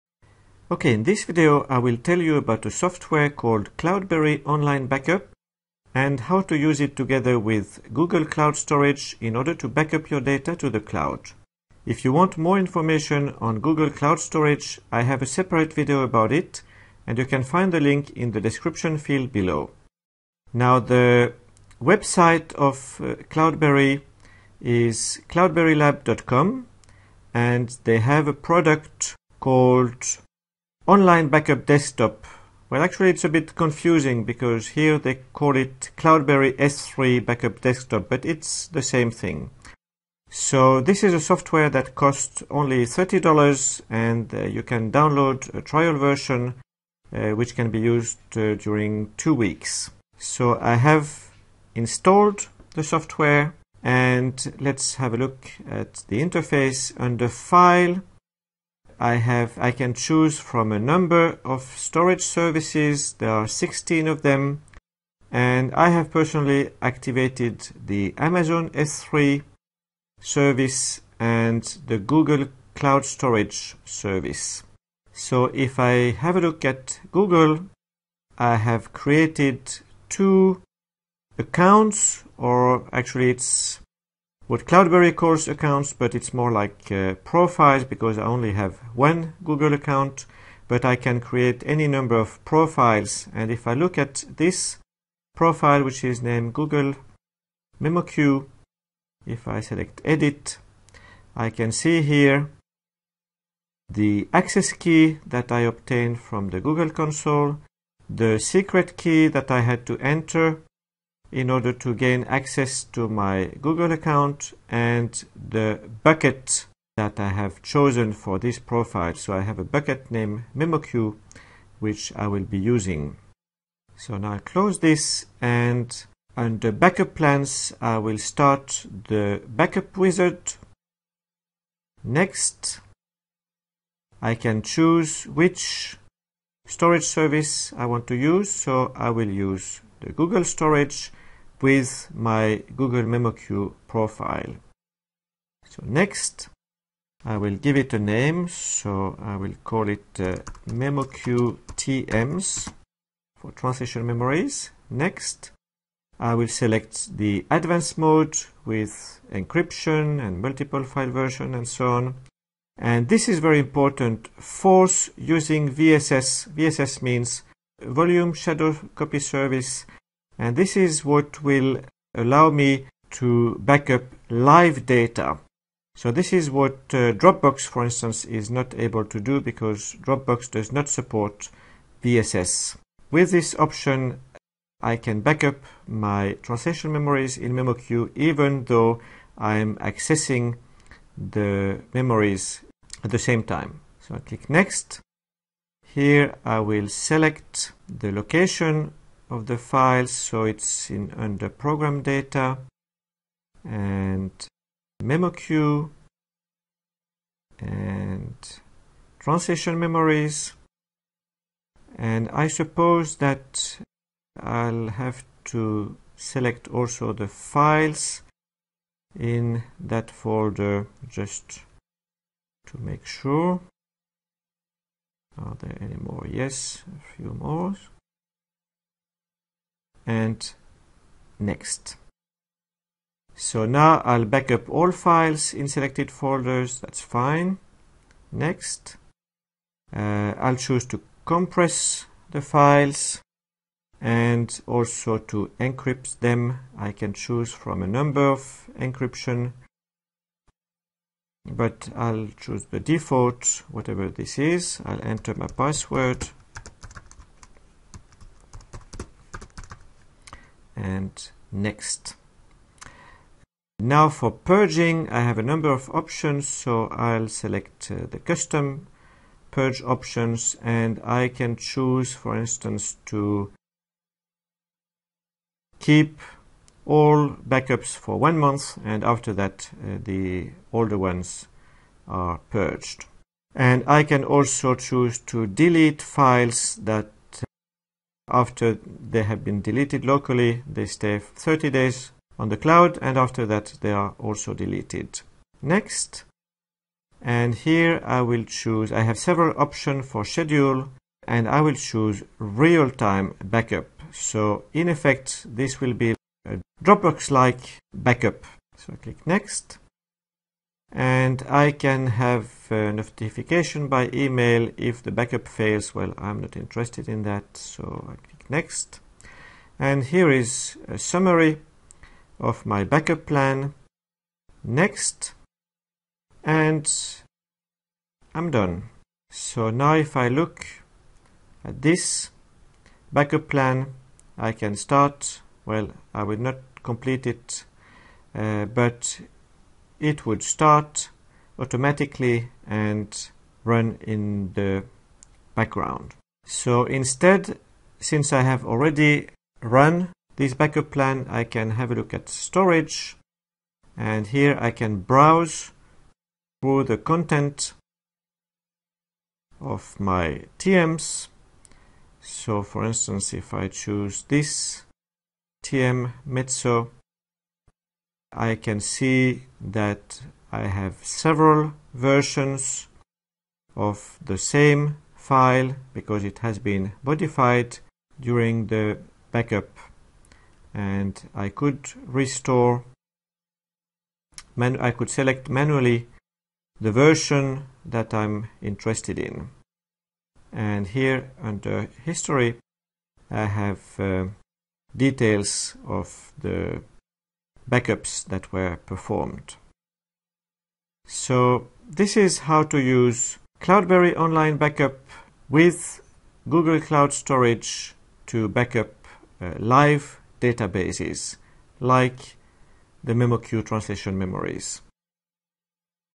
CloudBerry Online Backup with Google Cloud Storage
How to use CloudBerry Online Backup with Google Cloud Storage to backup live databases (memoQ translation memories).